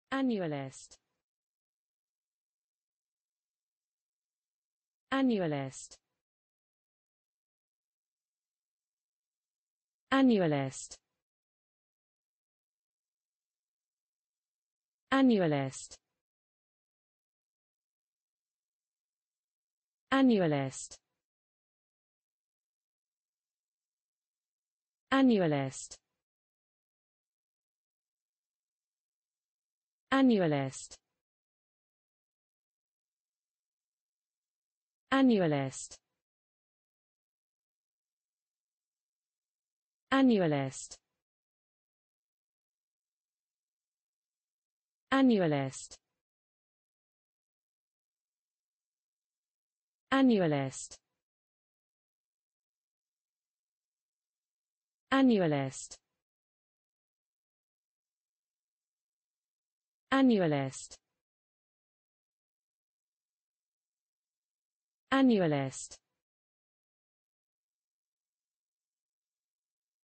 Annualist: How to pronounce Annualist in english (correct!).Start with A. Learn from me.
Follow me!Learn from me how to pronounce the word Annualist in english.This video shows you how to pronounce the word Annualist in english.

Annualist